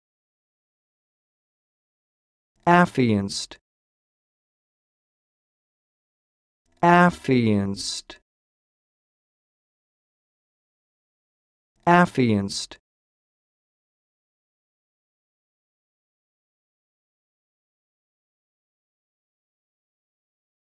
Learn how to say this word: "Affianced"
This is the definition of "Affianced":
imp. / p. p.  of Affiance